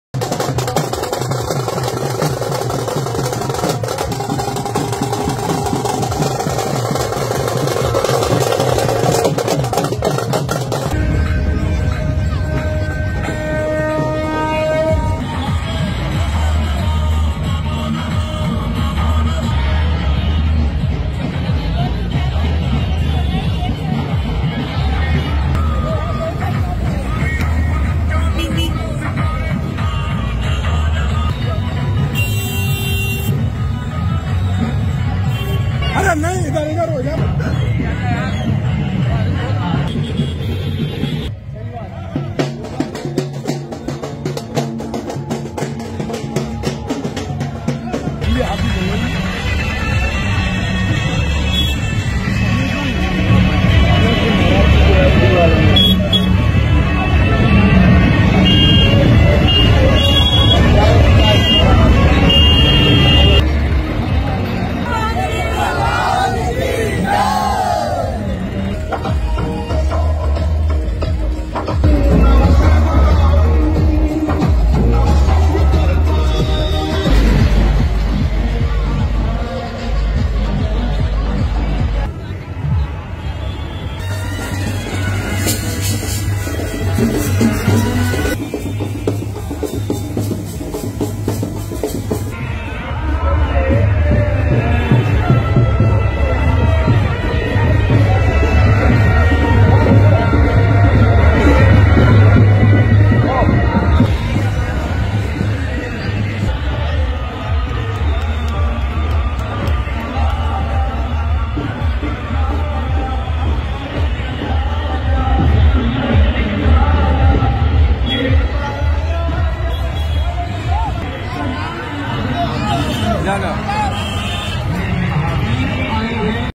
महावीर जयंती पर शोभा यात्रा का शुभारंभ विधायक ने किया-ATN NEWS UP
इटावा में महावीर जयंती के अवसर पर सदर विधायक सरिता भदौरिया ने शोभा यात्रा का किया शुभारंभ